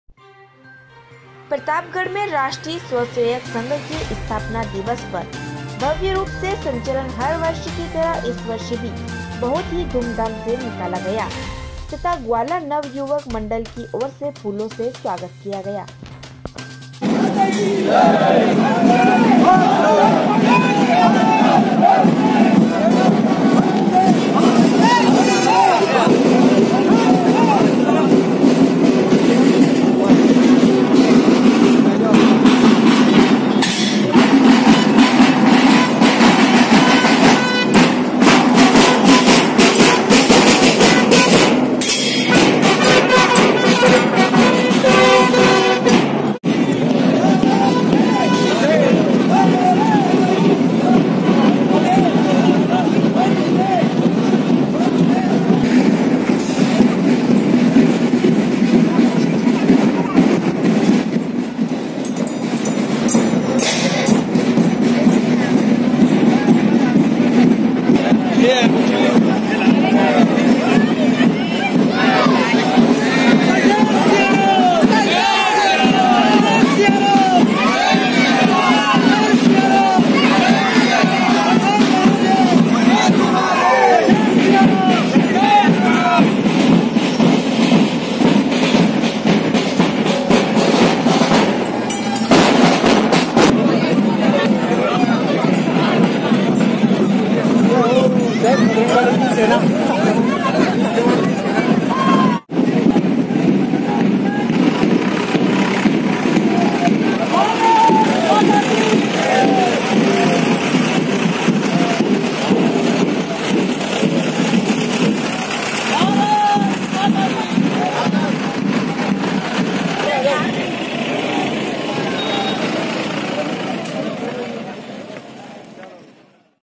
राष्ट्रीय स्वयंसेवक संघ के स्थापना दिवस पर भव्य रूप से निकला संचलन
प्रतापगढ़ (राजस्थान)